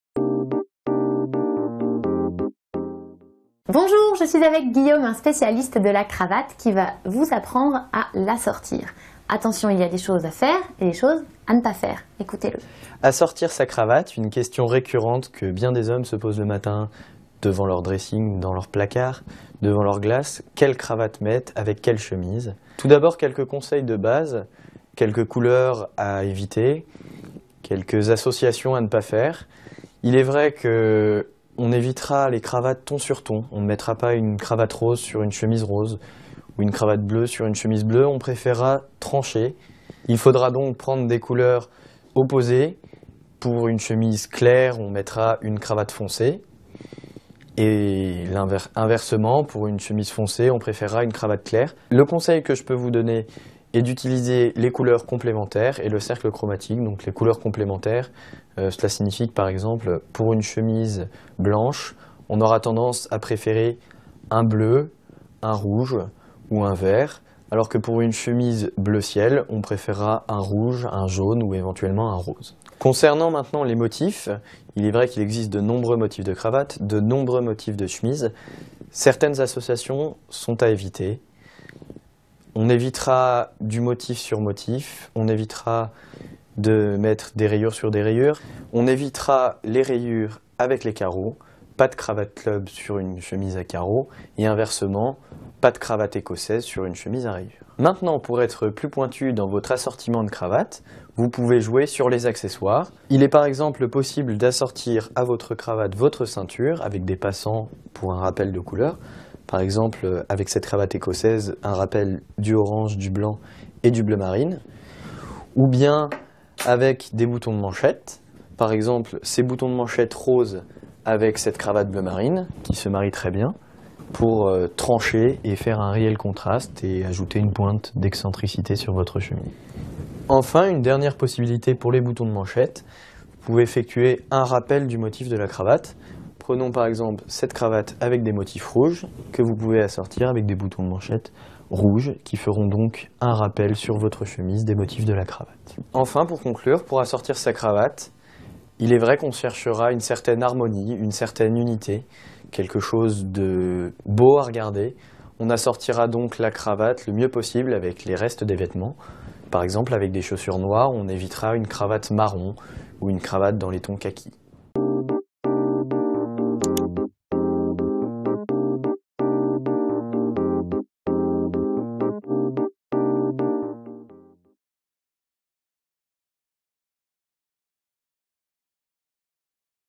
Astuces : Bien assortir sa cravate
Comment assortir sa cravate ? Voici les astuces à suivre pour assortir sa cravate.

Guillaume, un spécialiste de la cravate vous aide à assortir votre cravate.